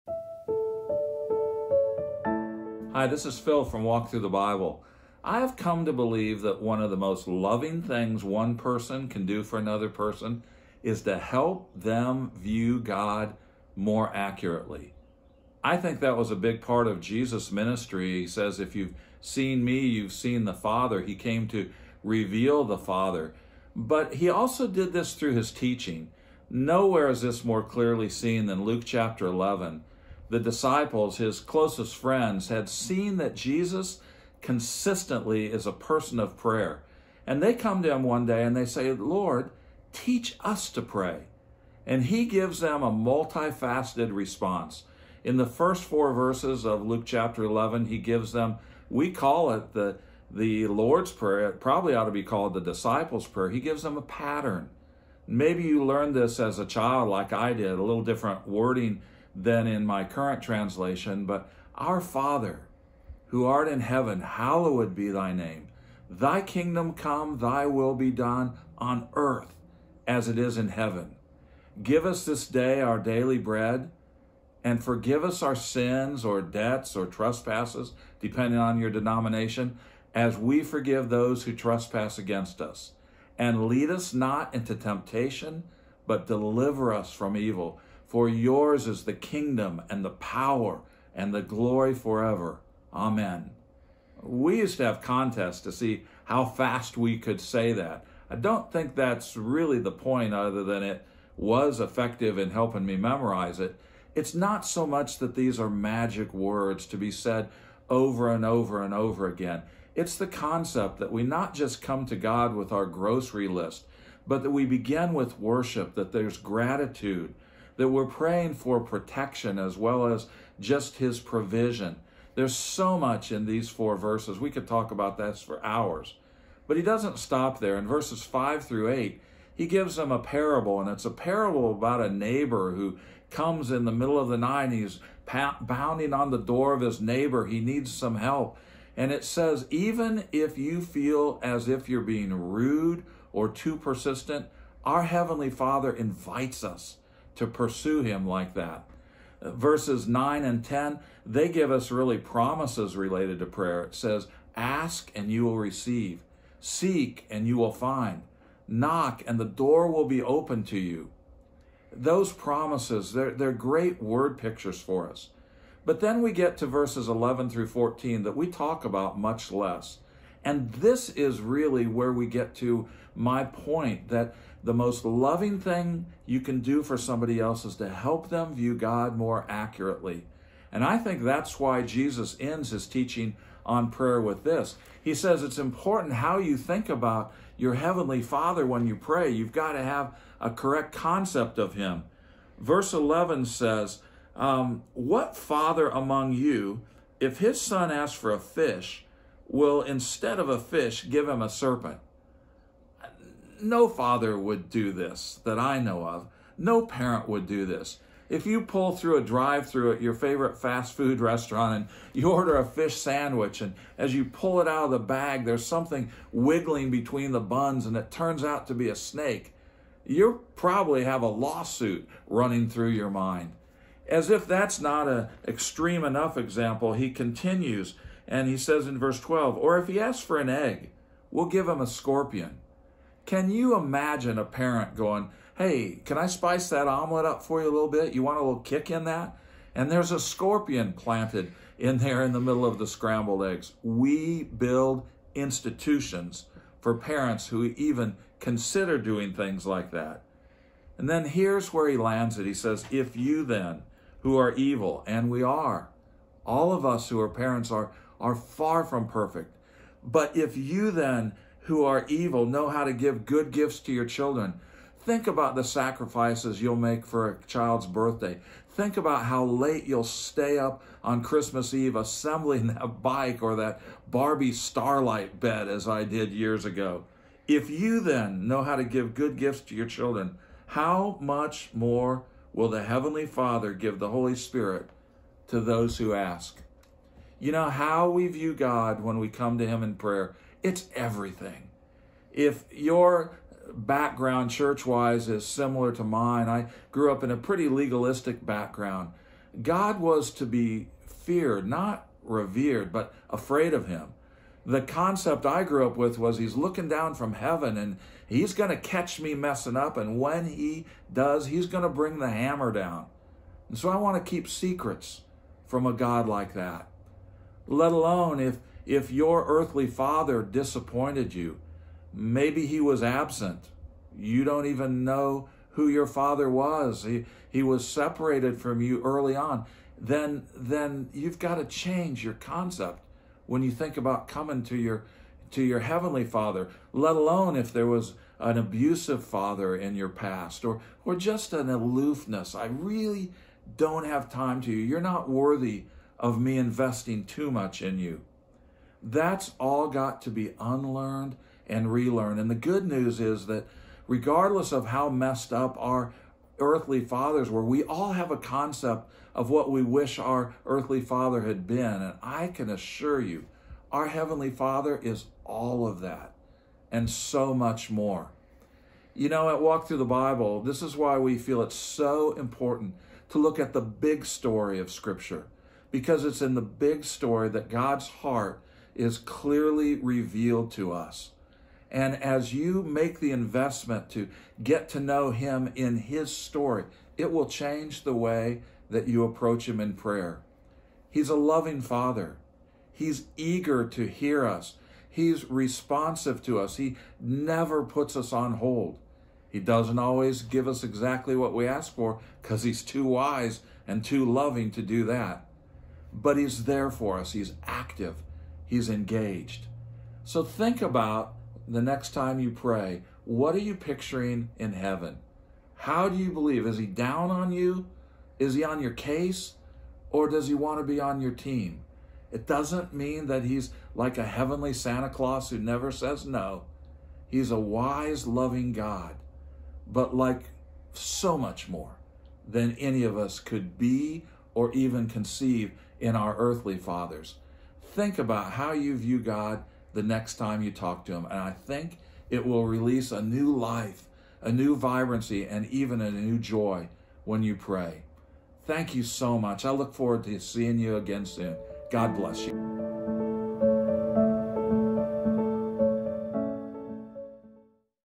WALK THRU VOICES: A Right View of God
"As you make the investment to get to know God in His Story, it will change the way you approach Him in prayer," says Phil Tuttle. "It's in the big story of Scripture that God's heart is clearly revealed to us." In this episode of WALK THRU VOICES, Phil talks about Luke 11, where the disciples ask Jesus to teach them how to pray (vv. 1-4); Jesus' parable of the persistent neighbor (vv. 5-8); the promises of prayer (vv. 9-10); and how having the correct view of God when we pray is everything (vv. 11-13).